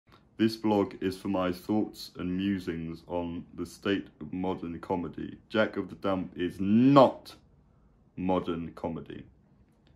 *REUPLOAD* Terry Collins IS ALIVE!
Terry Collins is still ALIVE. Taken from his since deleted Blog, he's seen talking about Jack Of The Dump.